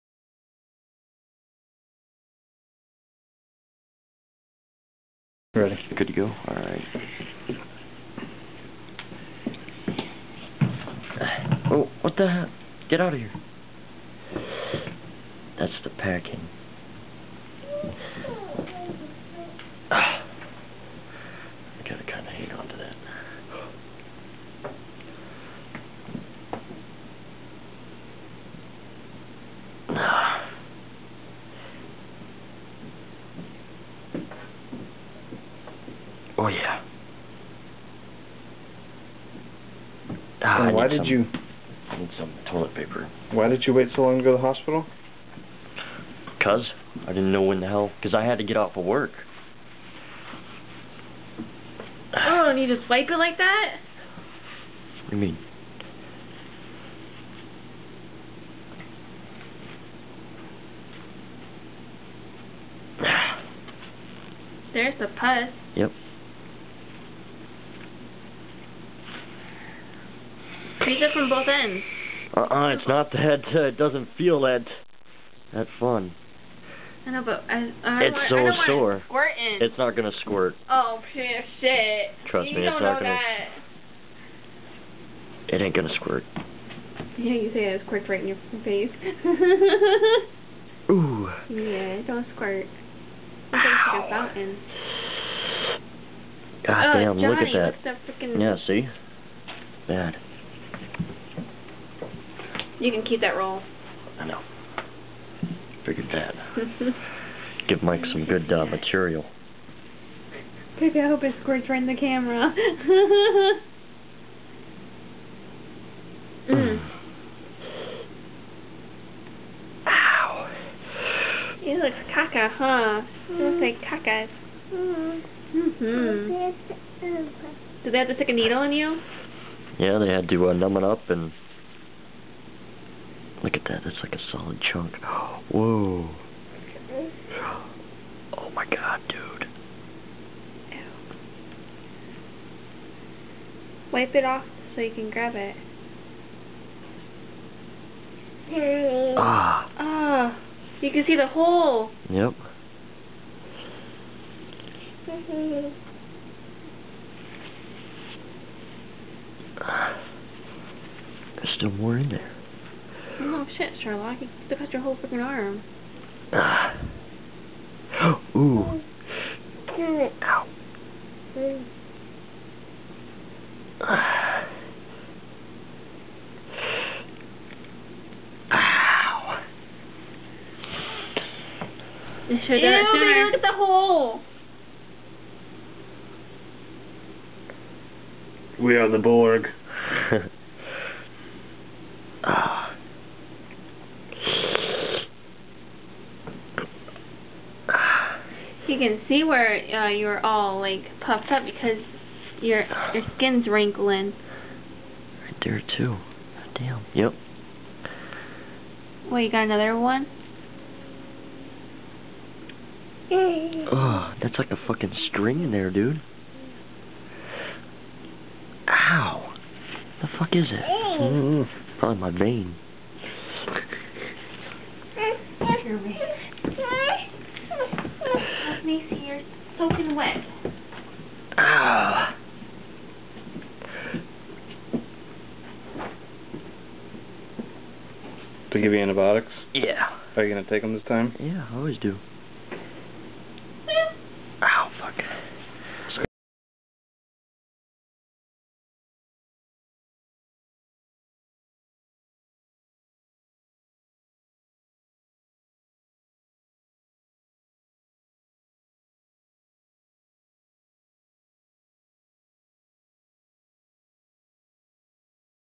Jonny's new Bite, pimple pop, cyst infection
Jonny was bit by something and had to drain his wound. The puss is overwhelming his arm... This massive infectious cyst almost cost Jonny his limb. Pimple popping to the gross. Shortly after this video, dead muscle and skin had to be trimmed away, leaving a gaping hole.